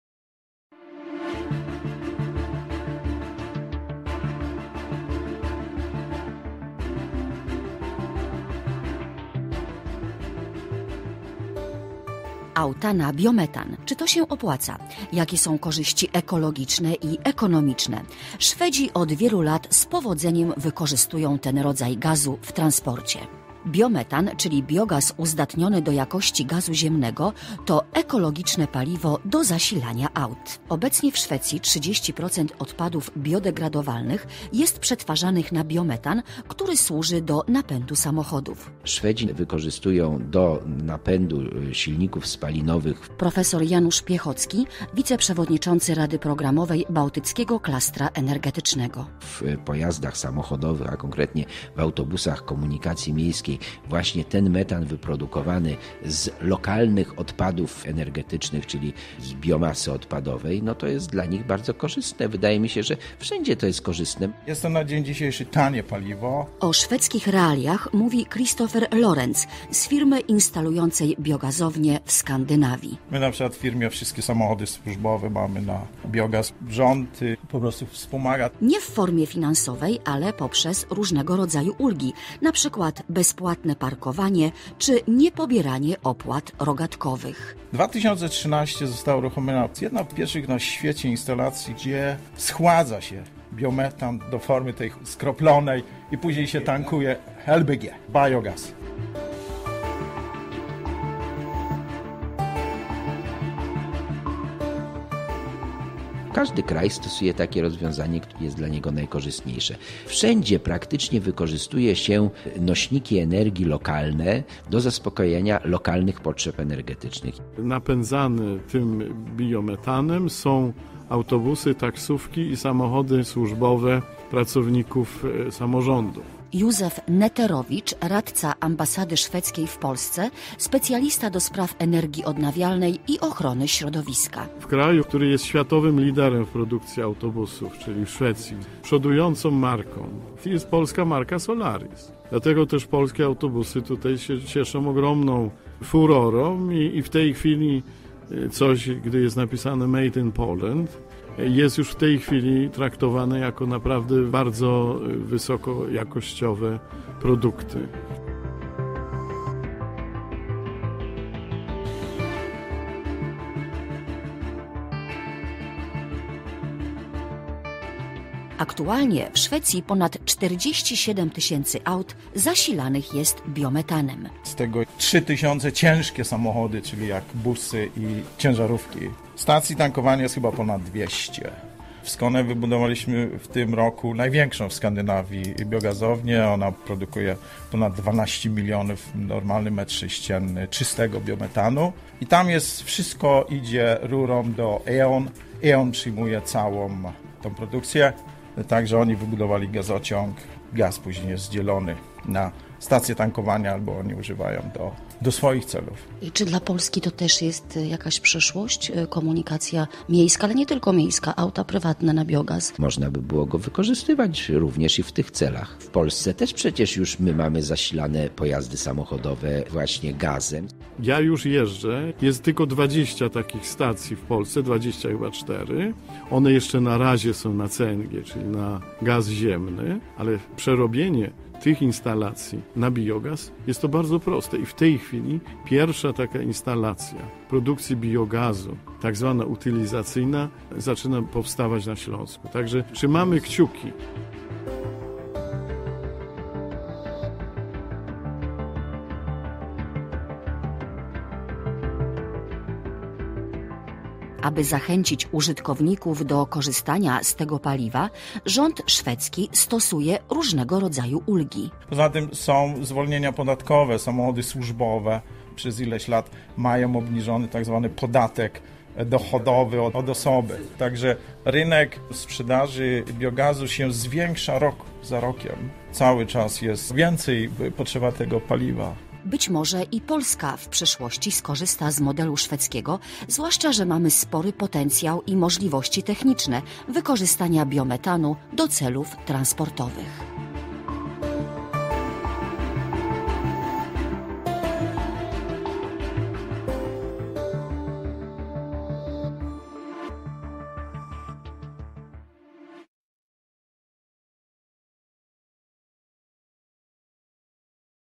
Auta na biometan
Czy samochód może być zasilany resztkami jedzenia, kiszonkami z traw i innymi odpadami biodegradowalnymi? Świat udowadnia, że tak… Materiał archiwalny z 2014 r.